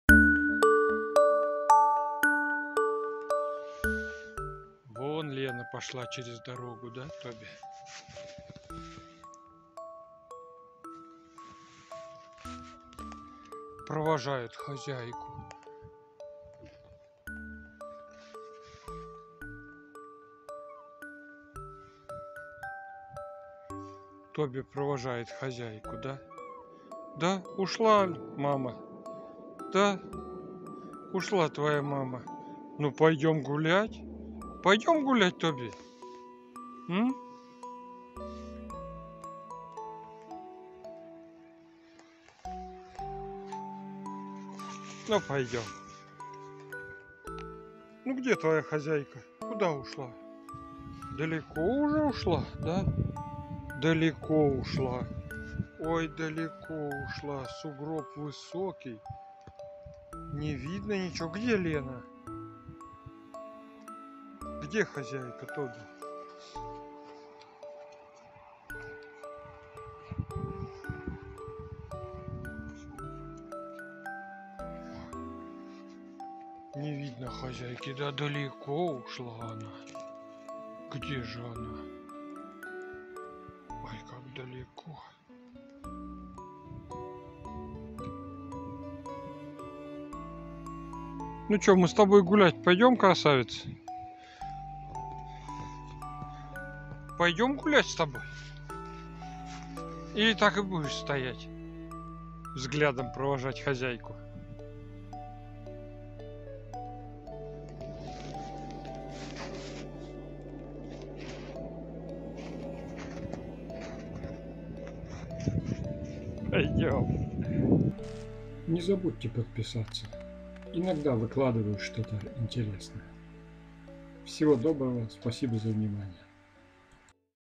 Цверг Тоби. Зайку бросила хозяйка...
Скучает... очень скачает Тоби по любимой хозяйке.